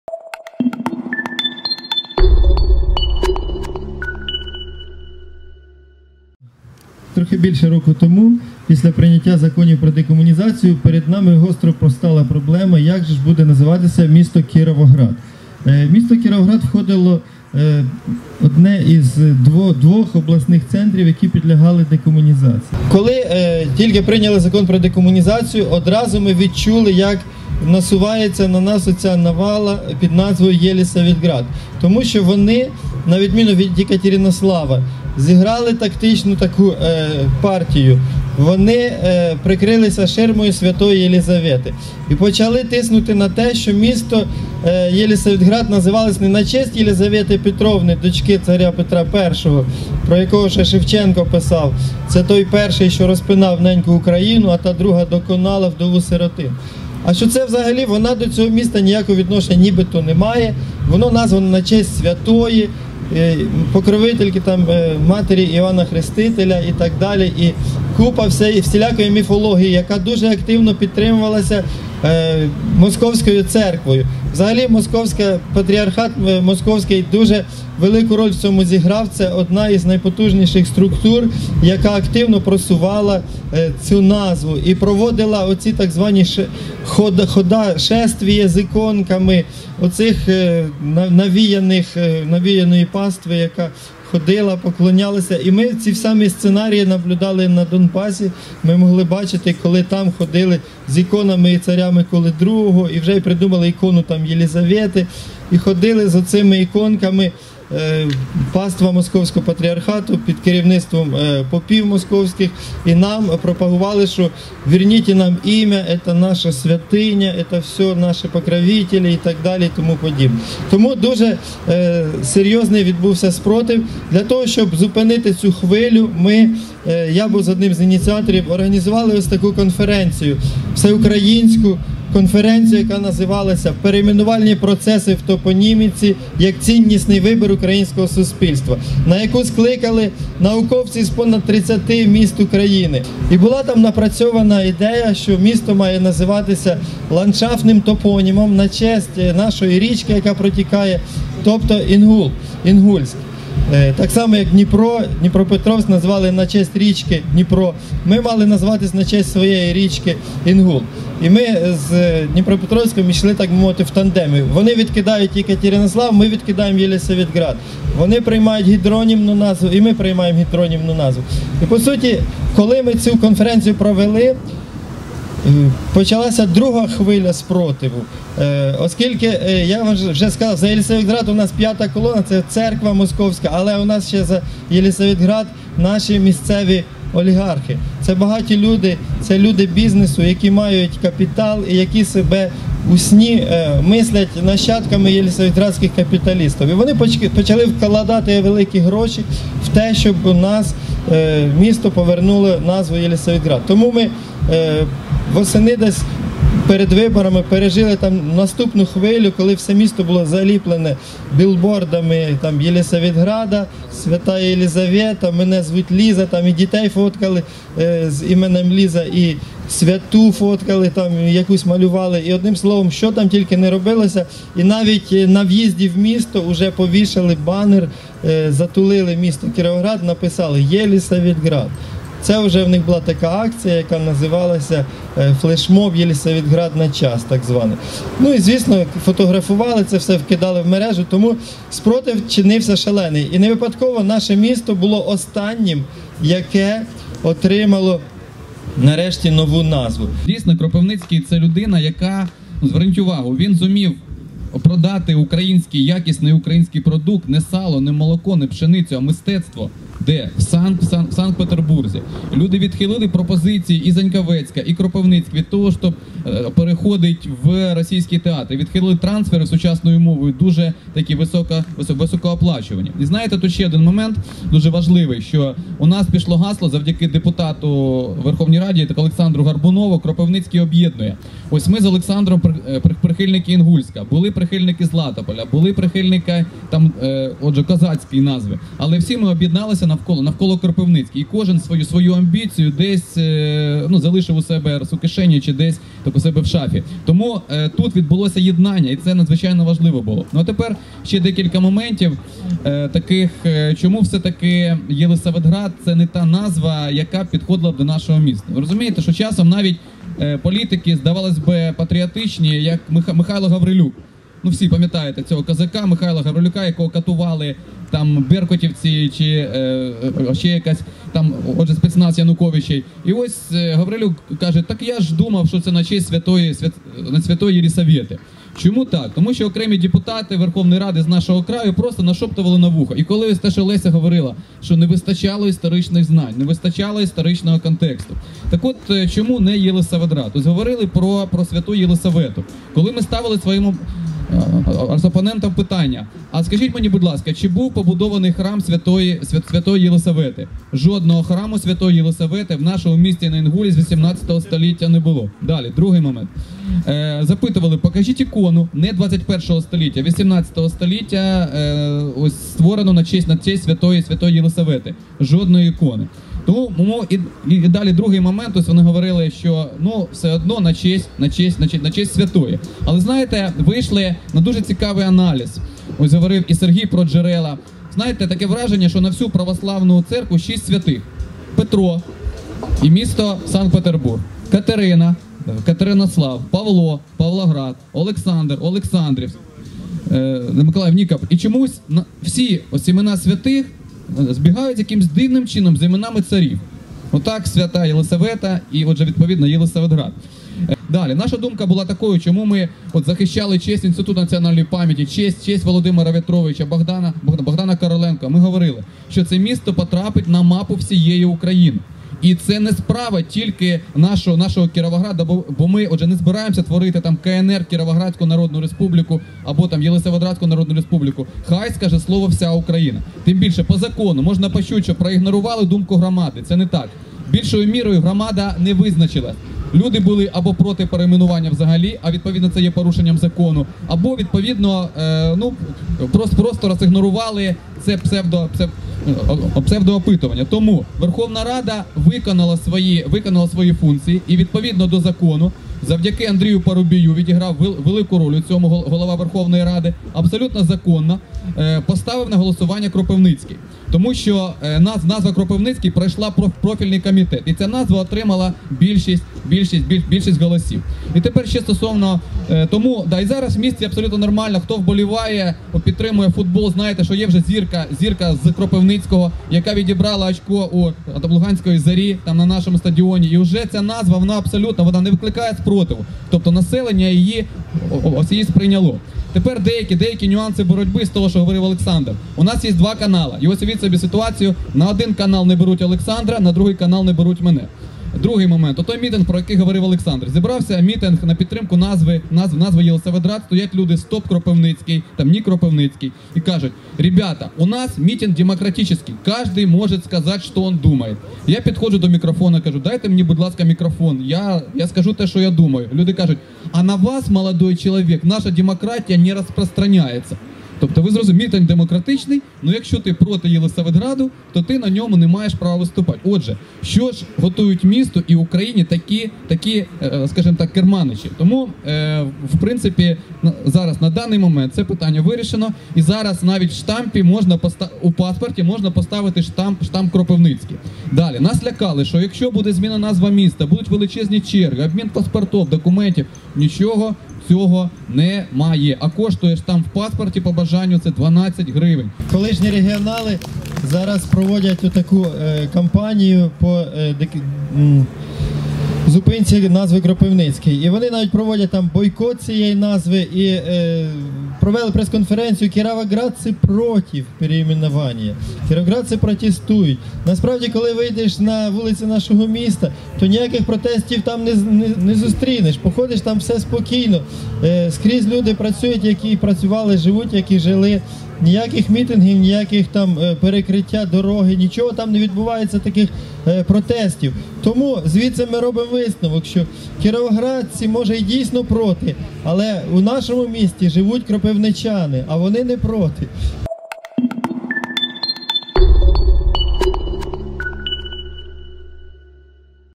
На Бандерштаті розповіли, як Кіровоград став Кропивницьким
Під час першого дня фестивалю «Бандерштат - 2016» на гутірці присвяченій темі декомунізації науковці з колишнього Кіровограду розповіли, як він став Кропивницьким.